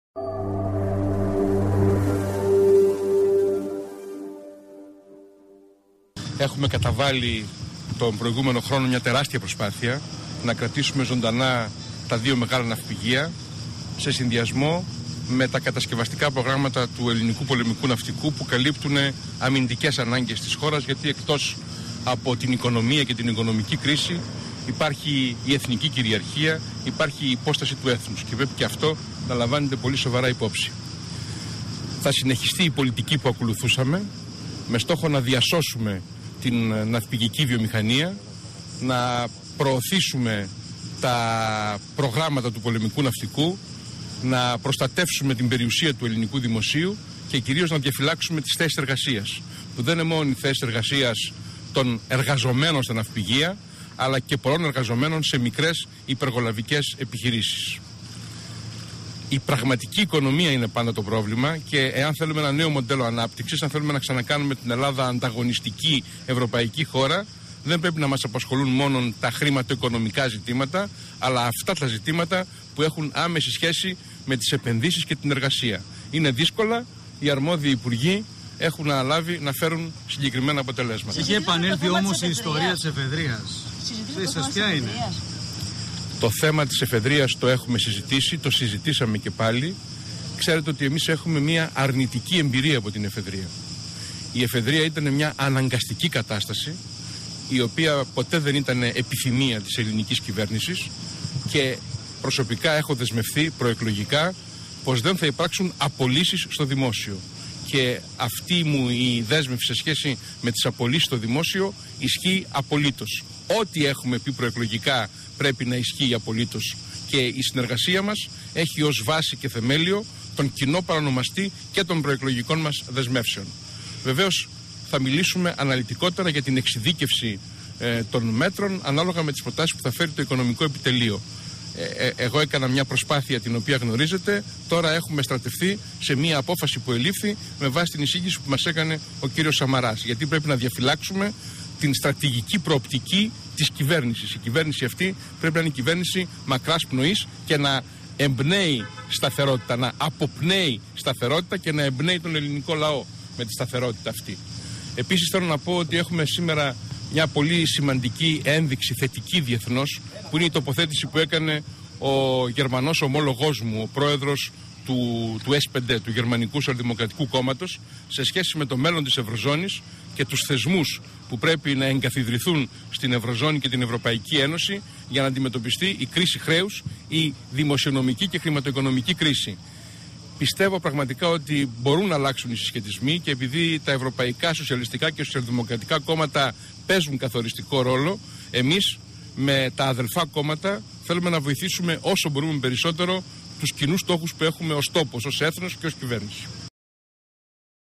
«Δεν θα υπάρξουν απολύσεις στο Δημόσιο»
«Ο στόχος είναι να διασώσουμε τη ναυπηγική βιομηχανία», δήλωσε ο πρόεδρος του ΠΑΣΟΚ, Ευάγγελος Βενιζέλος μετά τη συνάντηση των πολιτικών αρχηγών που συμμετέχουν στην κυβέρνηση στο Μέγαρο Μαξίμου. Ο κ. Βενιζέλος δήλωσε ακόμα ότι δεν θα υπάρξουν απολύσεις στο Δημόσιο.